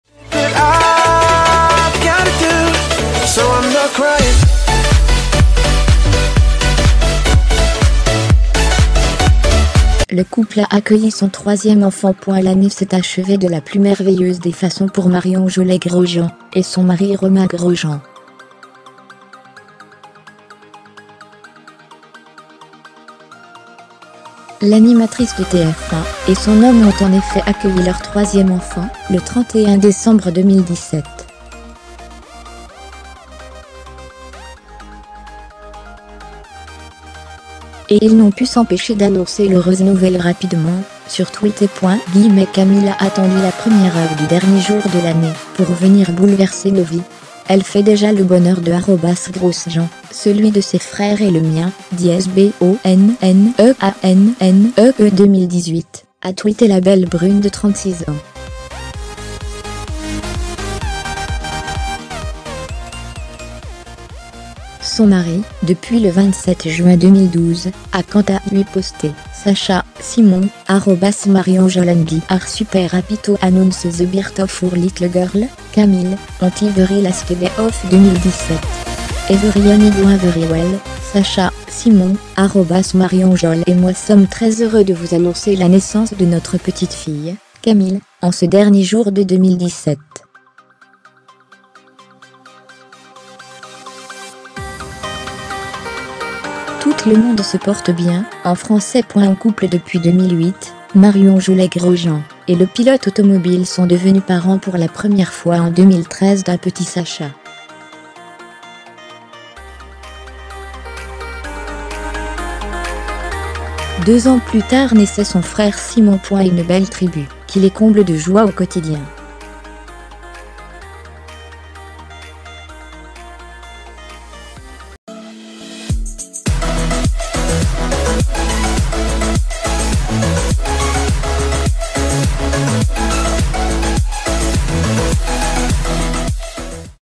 Marion Jollès-Grosjean maman : La femme de Romain Grosjean a accouché
Le couple a accueilli son troisième enfant.
L'année s'est achevée de la plus merveilleuse des façons pour Marion Jollès-Grojean et son mari Romain Grosjean. L'animatrice de TF1 et son homme ont en effet accueilli leur troisième enfant le 31 décembre 2017.
Et ils n'ont pu s'empêcher d'annoncer l'heureuse nouvelle rapidement sur Twitter. "Camille a attendu la première heure du dernier jour de l'année pour venir bouleverser nos vies ! Elle fait déjà le bonheur de @RGrosjean, celui de ses frères et le mien BonneAnnee2018", a tweeté la belle brune de 36 ans. Son mari, depuis le 27 juin 2012, a quant à lui posté : "Sacha, Simon, @marionjolles and I are super happy to announce the birth of our little girl, Camille, on this very last day of 2017! Everyone is doing very well" ("Sacha, Simon, @marionjolles et moi somme très heureux de vous annoncer la naissance de notre petite fille, Camille, en ce dernier jour de 2017. Toute le monde se porte bien", en français).
En couple depuis 2008, Marion Jollès-Grosjean et le pilote automobile sont devenus parents pour la première fois en 2013 d'un petit Sacha. Deux ans plus tard naissait son frère Simon. Une belle tribu qui les comble de joie au quotidien.